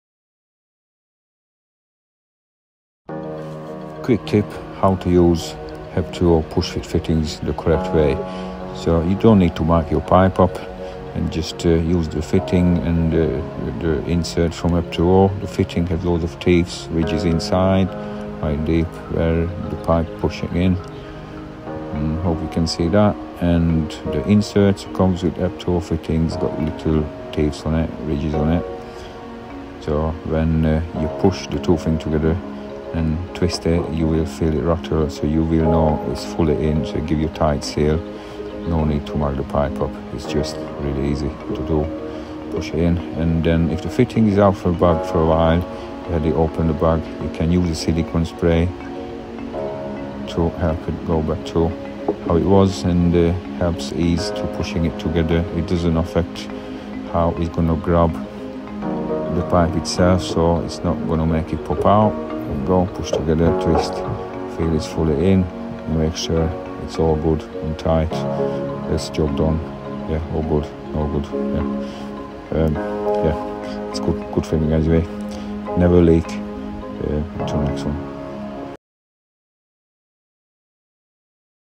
Avoid Leaks! Correct Way to Use Hep2O Fittings
In this video, I show you the proper way to use Hep2O push-fit plumbing fittings. Whether you're new to plumbing or an experienced installer, getting that tight seal is crucial, and you don’t always need a marker line if you know what to feel for.

I’m a Leeds-based gas engineer, and this is how I make sure every fitting is seated properly – first time.

🔧 No leaks. No call-backs. Just solid installs.